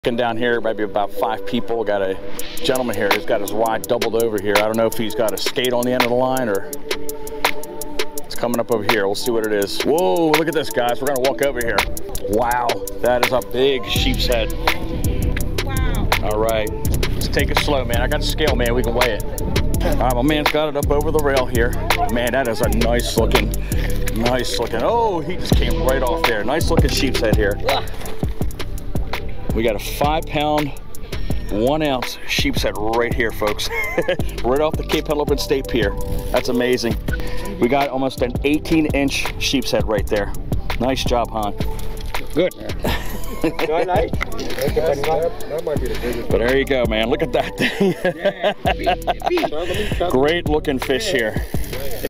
Big Sheepshead Fishing
Big Sheepshead Fishing off the Cape Henlopen Pier in Lewes, DE pierfishing

Cameras:

📷 GOPRO 7 Black (2)
📷 GOPRO 8 (Go Pro Media Mod)

Microphone:

🎤EDUTIGE ETM-001 Microphone - Omnidirectional 3.5mm 3-Pole(TRS) Microphone for GoPro